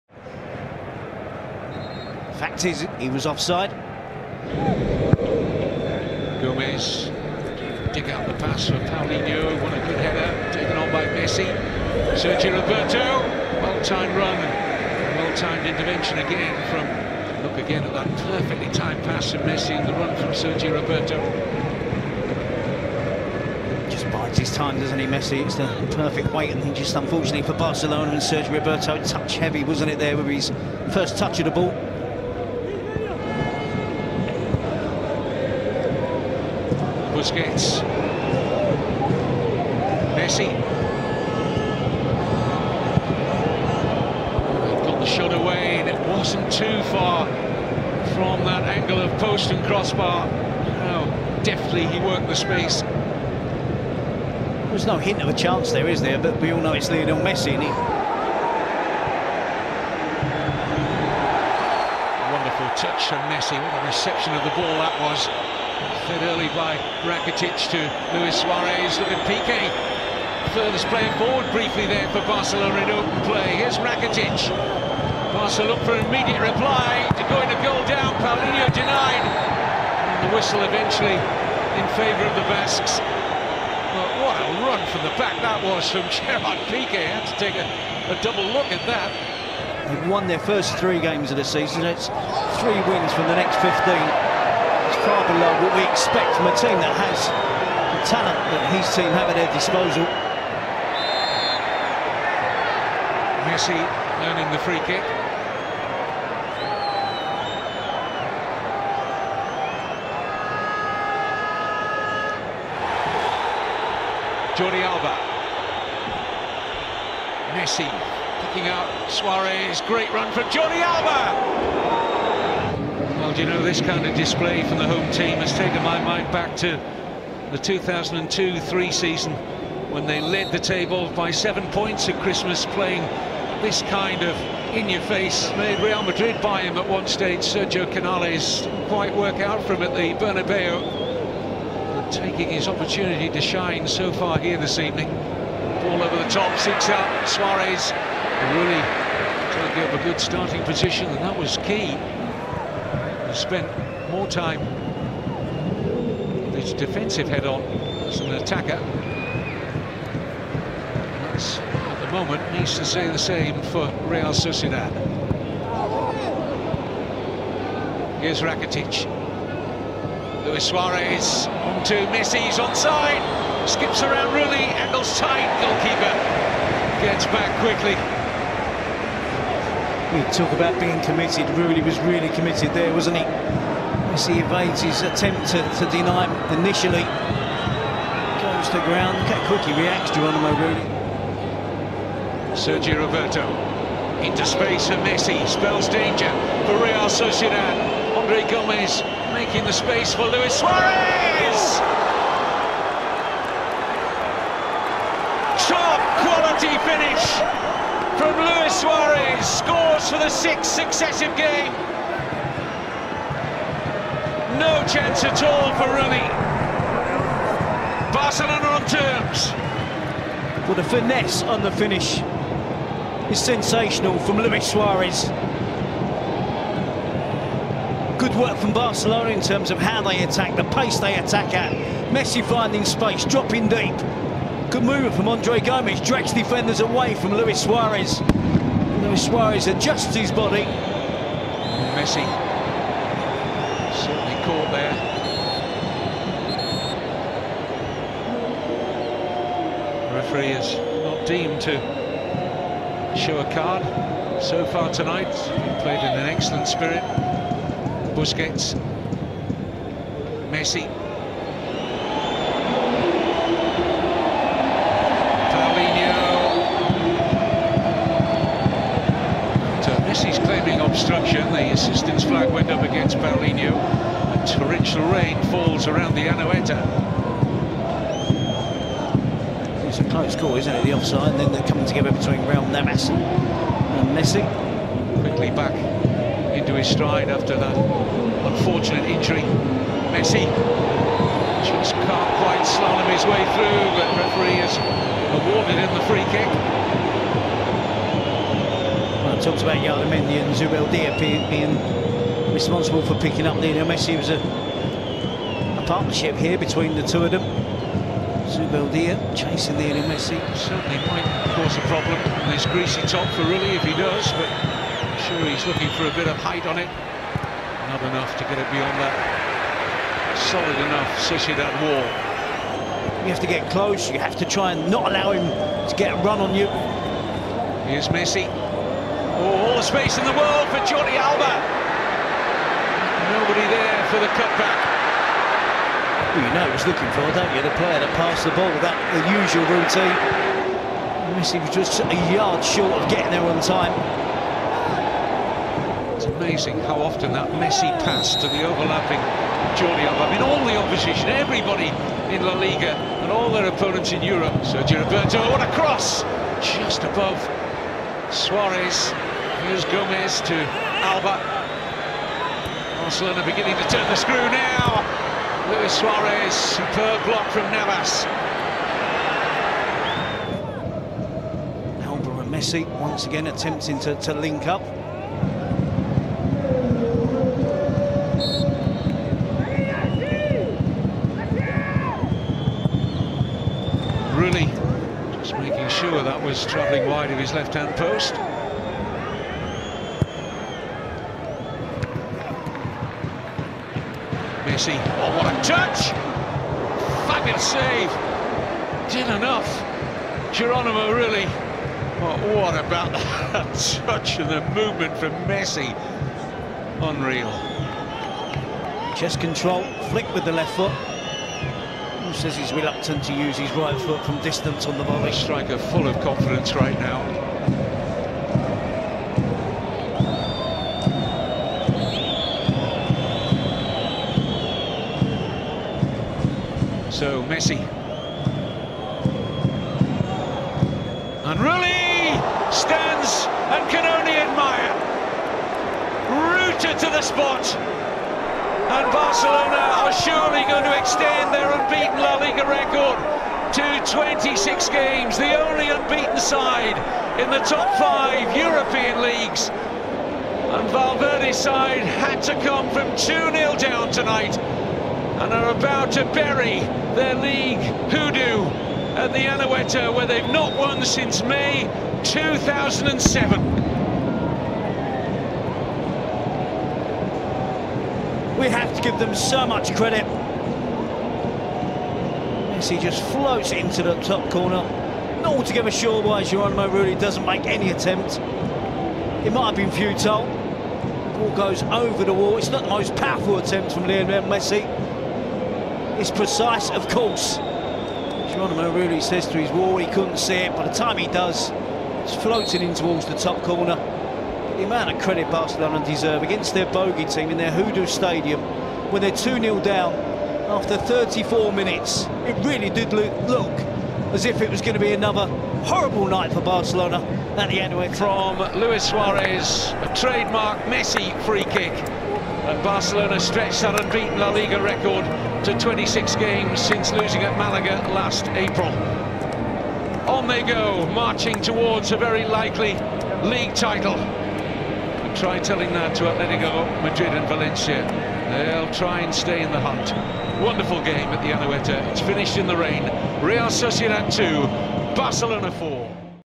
Lionel Messi vs Real Sociedad (Away) 17-18 HD 720p (14/01/2018) - English Commentary
English Commentary.
La Liga 2017-18.
By EdenHazard10i.

Real Sociedad 2-4 Barcelona.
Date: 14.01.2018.